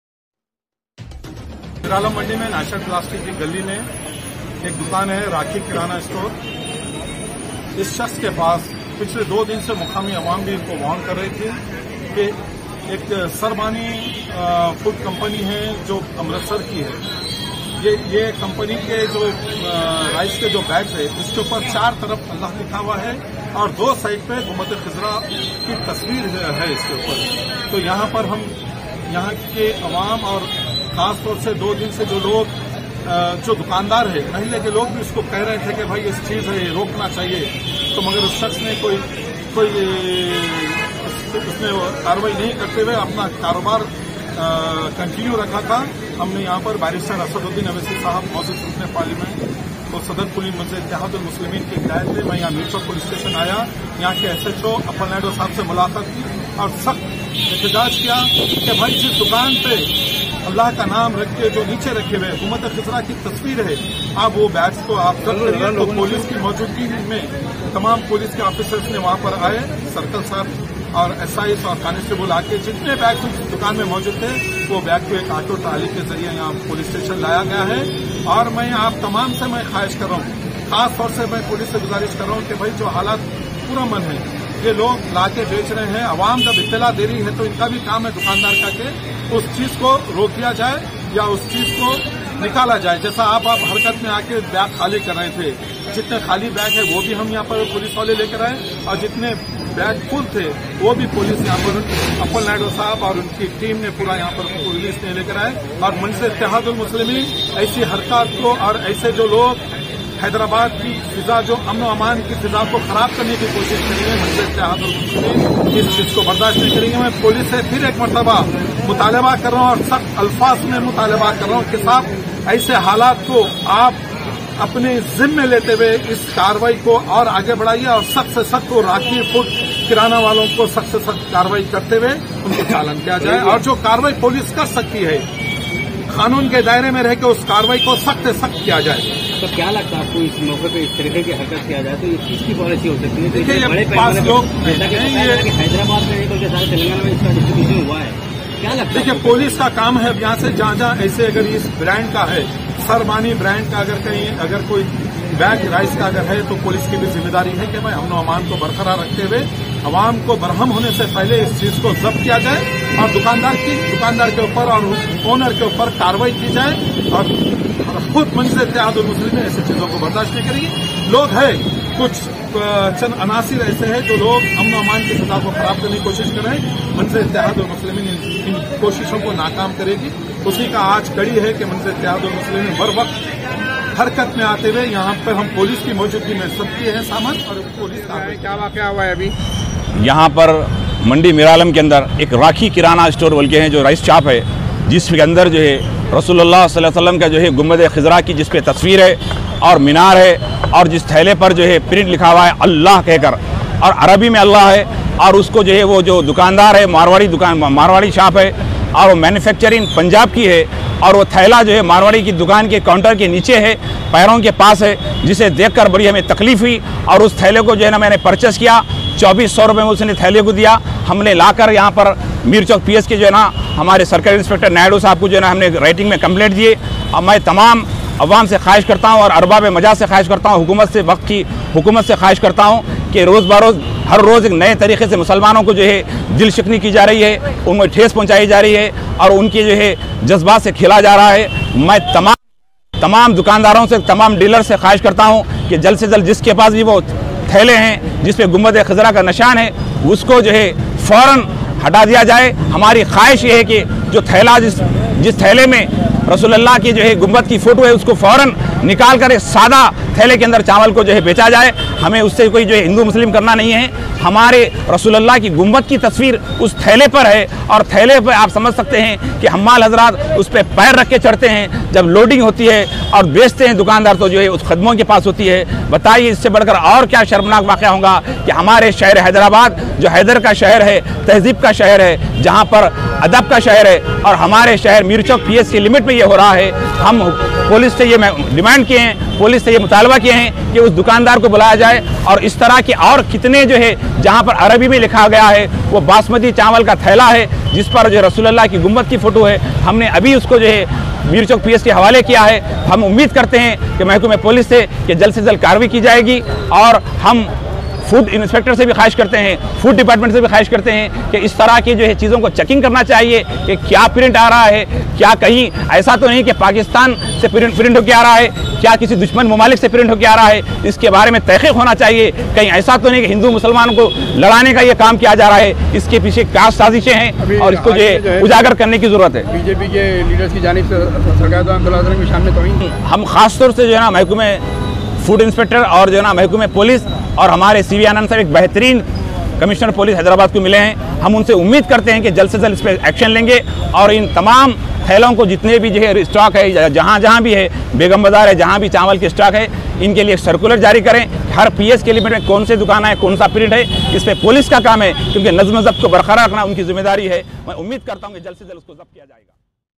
Aaj tanzeem e islahe muwashera aur social ki janib se Meer chowk police station me ek complain ke
Aaj tanzeem e islahe muwashera aur social ki janib se Meer chowk police station me ek complain ke gaye.
Miralam mandi ke ek dukan RAKHEE kirana store me Chawal ke thaile (Rice bag ) par ALLAHA aur Gumbad e khazra ki tasaveer payee gayee Social Media k janib se guzishta 3 din se tawaj'jo dilane par bhi koi bhi action nahi lene par tanzeem e islahe Muwashera ke HABEEB RAHMANI Mir chowk police station me ek complain kya.aur  social media aur police ke Harrah Chawal ke thailo (RICE BAGS) ko zapt karwaya